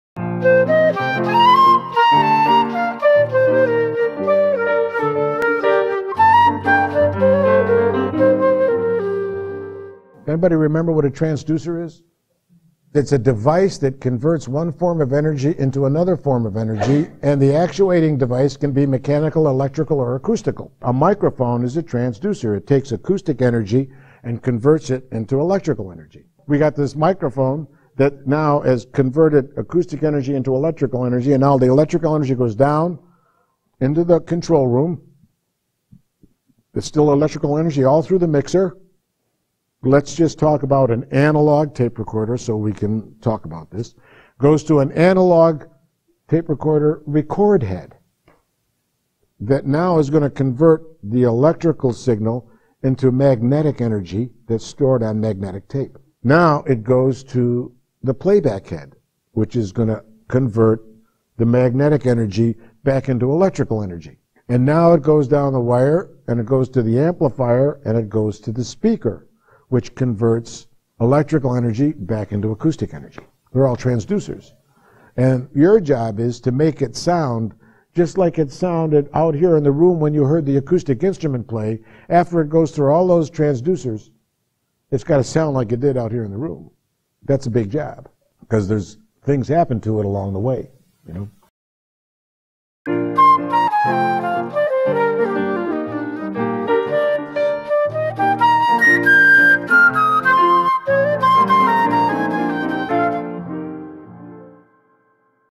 What is a Transducer?
"What is a Transducer?" is a lecture video from the textbook Basic Audio Recording Techniques, taught by Mike Julian at Los Angeles Valley College.